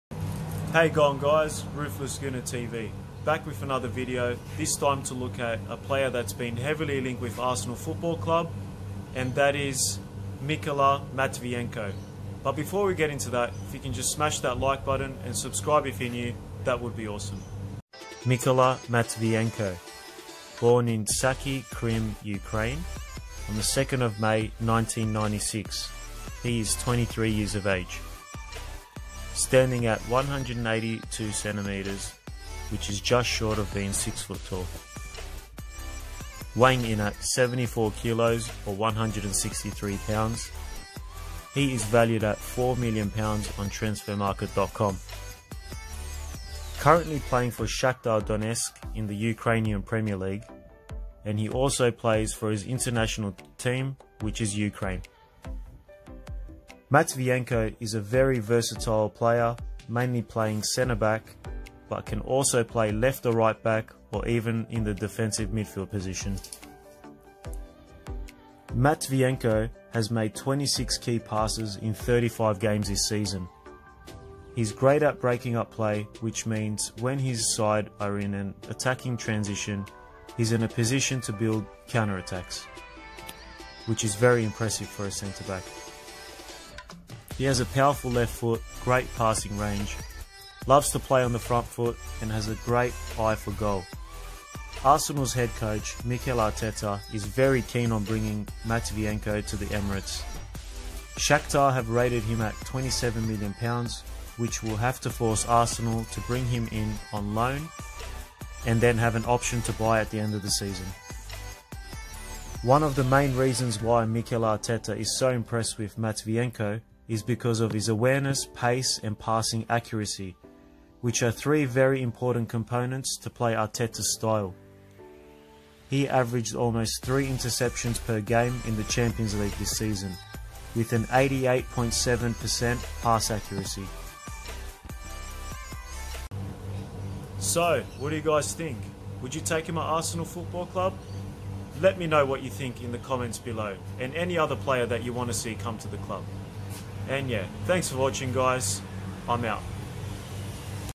Mykola Matviyenko To Arsenal? Transfer News Today | Player Breakdown | Ruthless Gooner T.V
lets take a look at a player that has been linked with Arsenal football club, Mykola Matviyenko